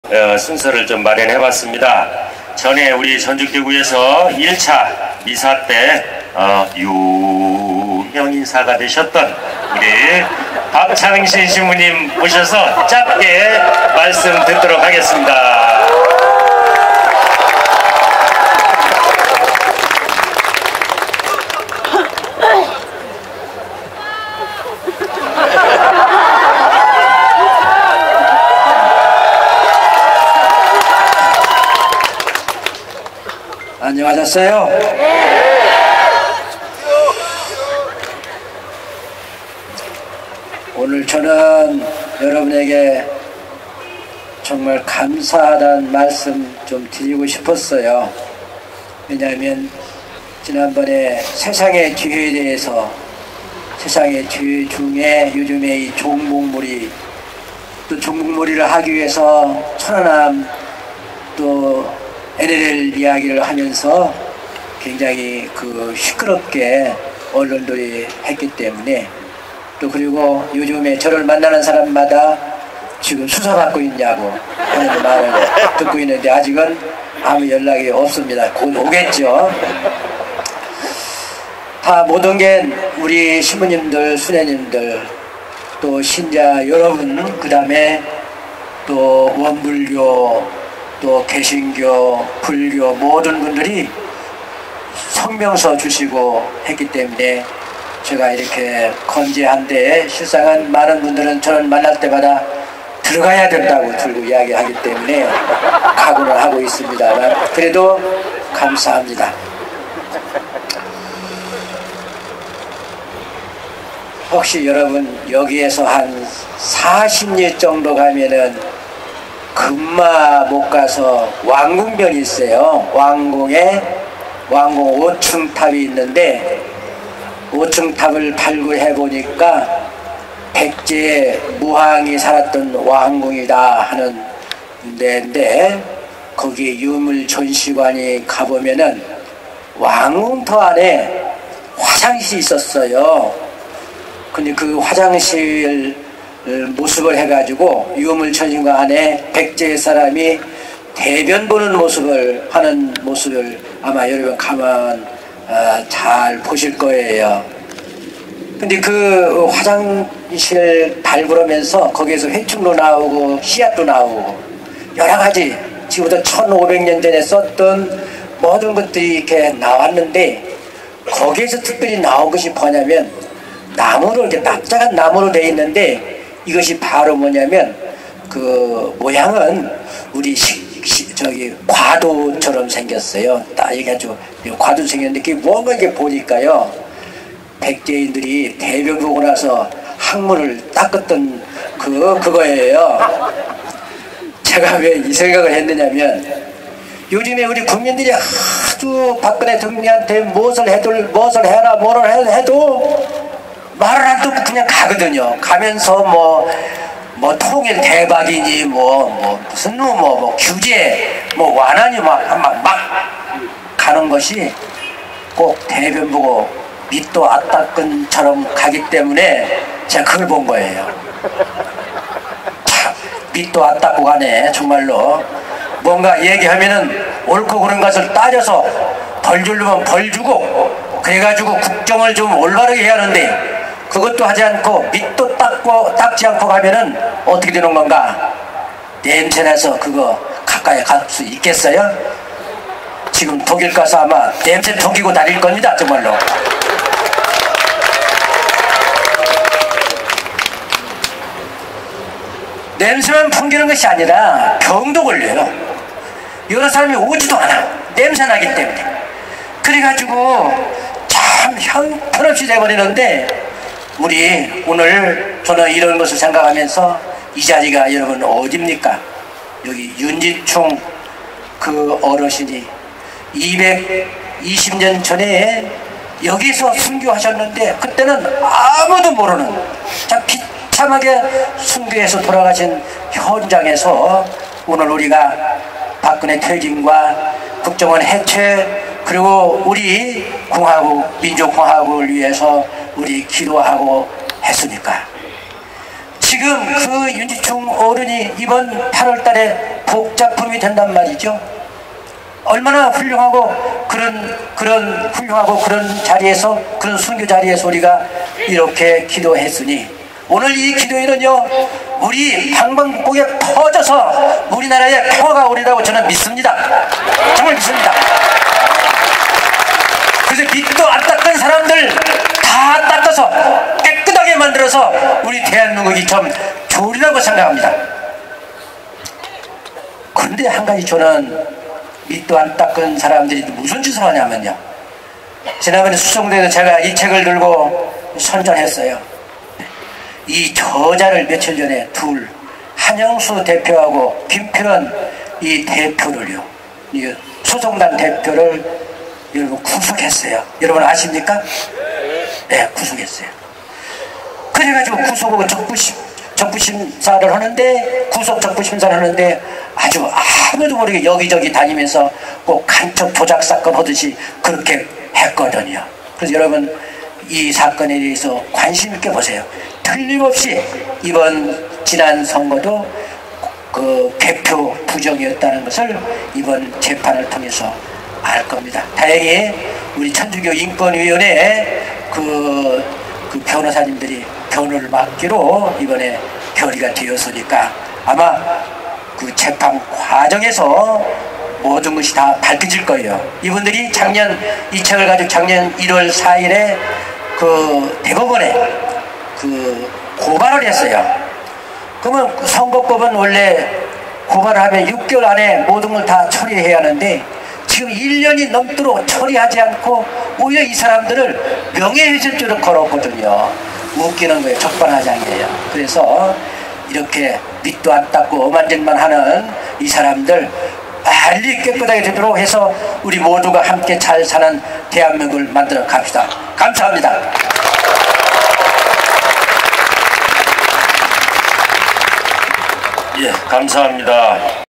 정구사 신부 박창신, 대통령 희롱: "대변 보고 밑도 안 닦고 다닌다"
천주교 전주교구 원로신부. 2014년 3월24일 저녁 7시부터 전주 풍남문 광장.